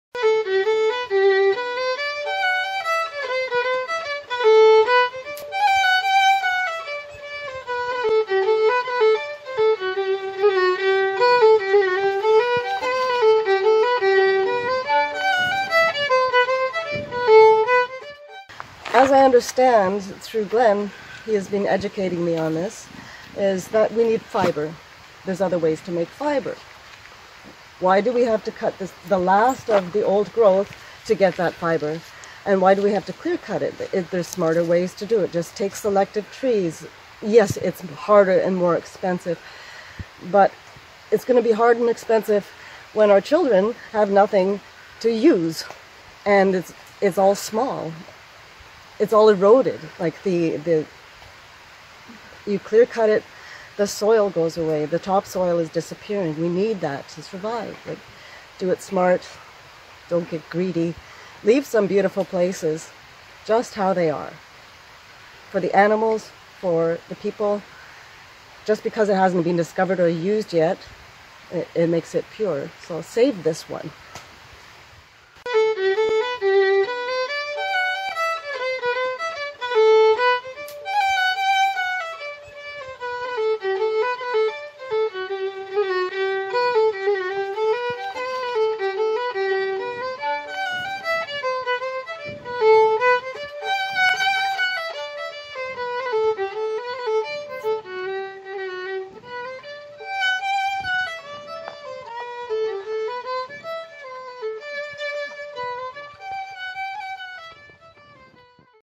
Lillian Penner: Why Do We Cut The Last Old Growth Forests Down?
At the Fairy Creek blockade, artists are visiting and sharing their gifts. One of these Lillian Penner was asked about what she thought about the last remaining old growth valley that Teal Jones wants to clearcut.